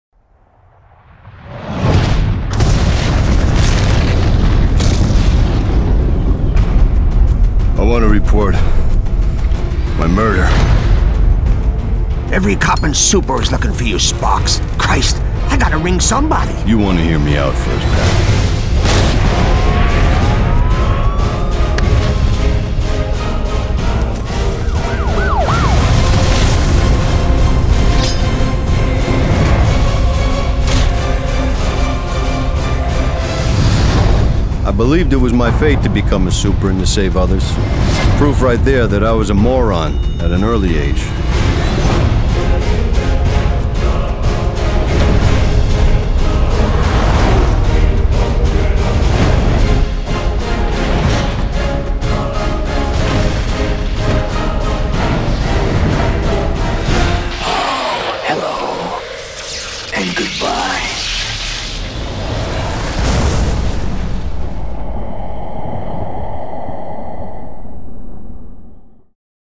William Katt's Sparks Trailer
This is the trailer for the iPhone Comic Book Sparks! Very cool!